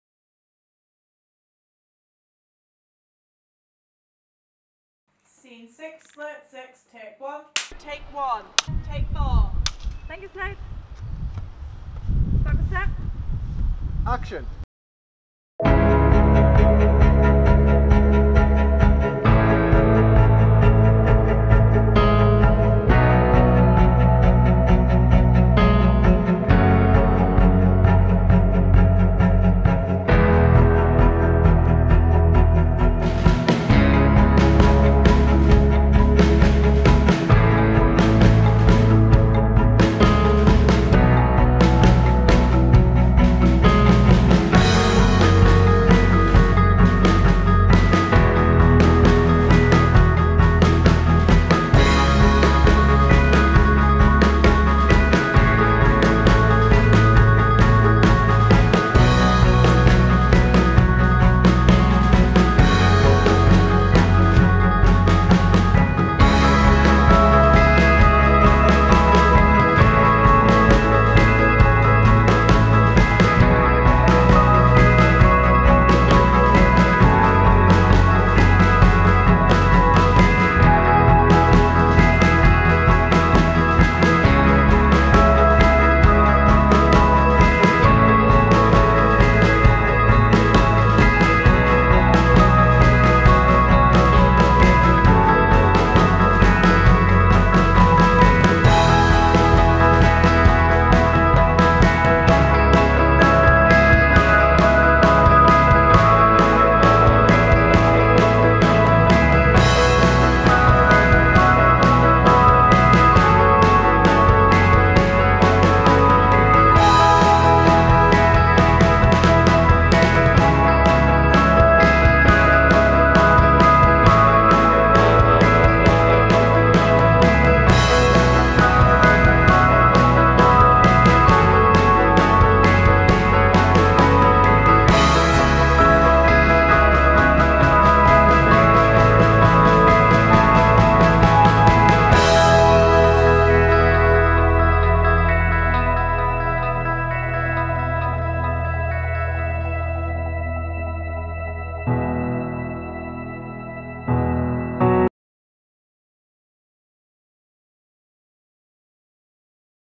Polarity - Behind the Scenes - My Rode Reel 2020
Behind the Scenes film for the My Rode Reel short film entry 'Polarity'

Filmed and Edited by Nathan Wood